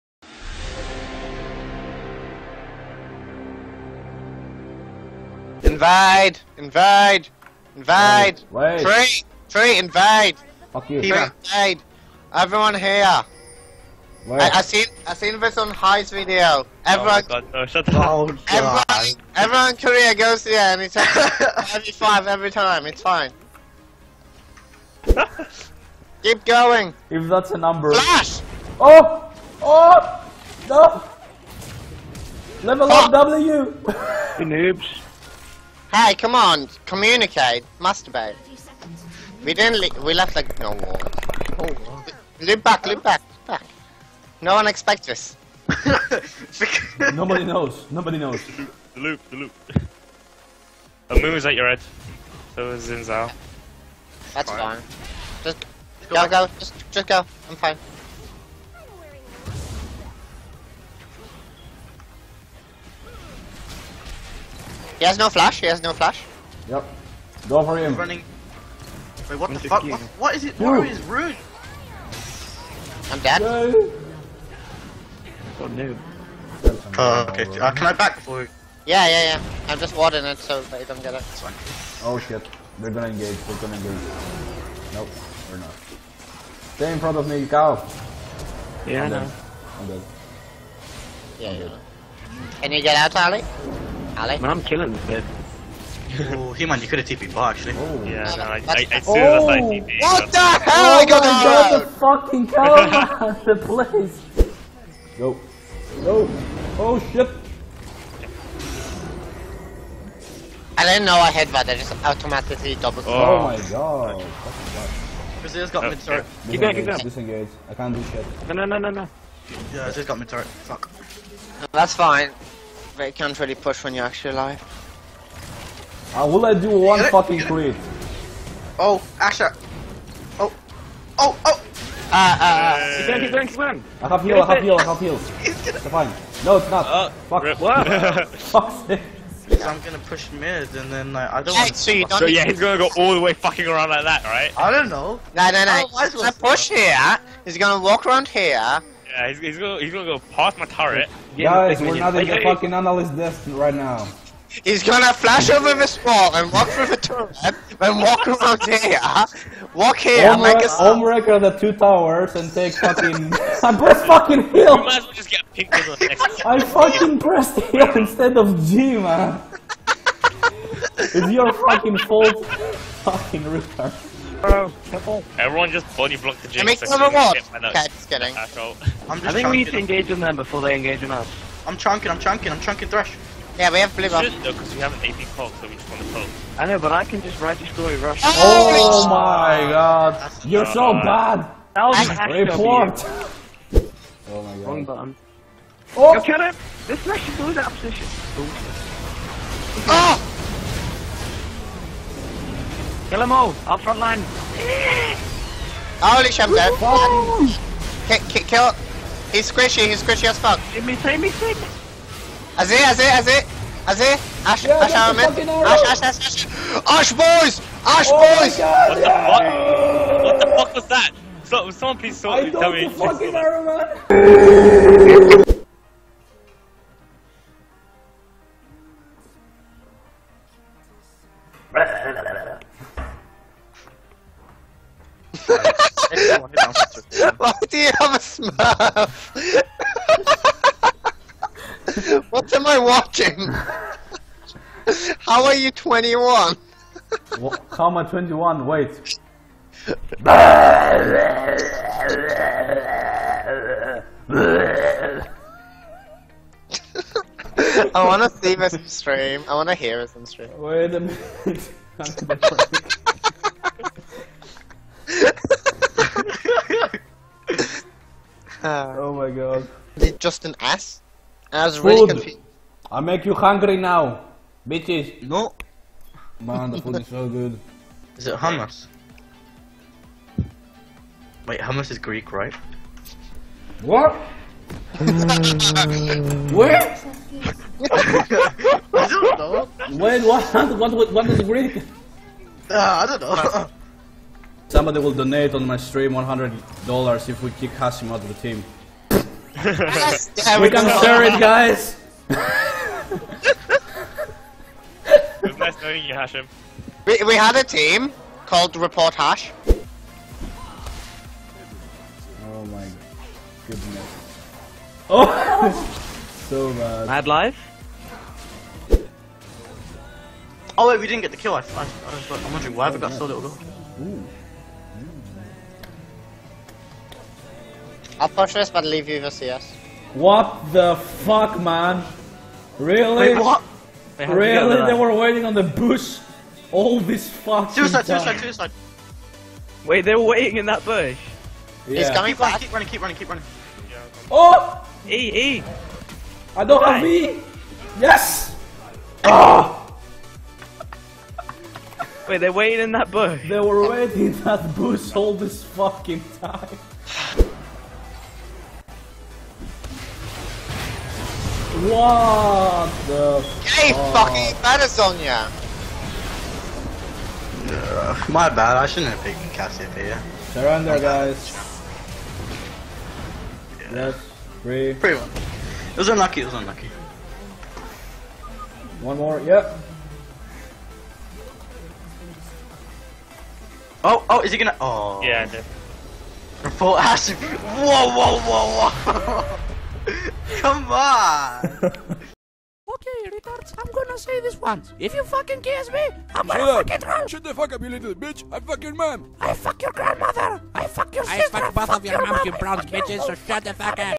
Stream Highlights & Funny Moments #1. Report Hashim, Hummus, MadCow
Hey guys this is the first video of this series so dont hate me :)
I really really apologise for the bad mic quality and image quality.
Since I don't have enough hard drive space to record the footage while streaming. (640 GB HDD)
Enjoy! :D

If you want to support me do the following (or I will kill you :D):